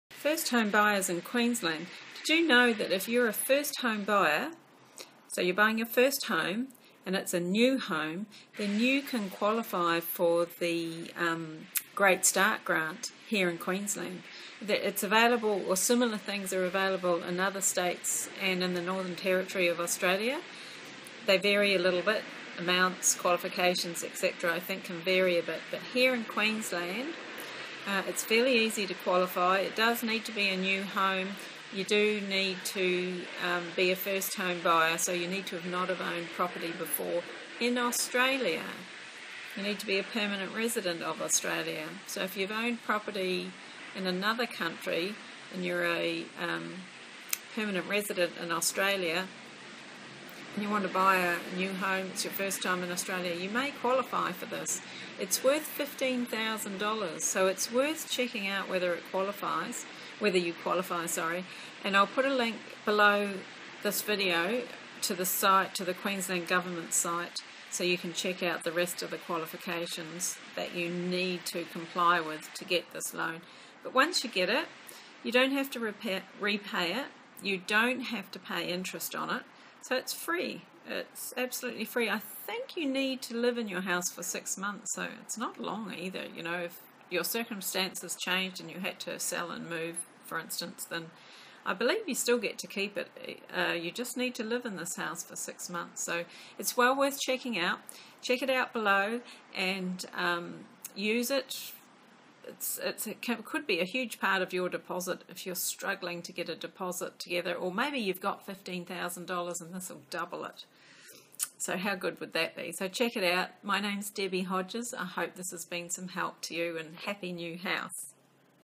Copy of First Home Buyers Qld - Buying New? Then You Need To Know This
First Home Buyers Qld - Buying New? Then You Need To Know This.
I hope that all First Home Buyers in Qld know about this and if you don't then keep reading because this may make you feel  a whole lot richer.
If you are a first home buyer and you are buying a new house then you may qualify to get the "Great Start Grant" which will put $15,000 into your new home and it's all from the Qld government.
There are a few things that you need to qualify and you can find out what they are
Get your mortgage lender or broker at apply for the Great Start Grant for you and get into your new home sooner.
Learn how to pay off your mortgage sooner - follow the link above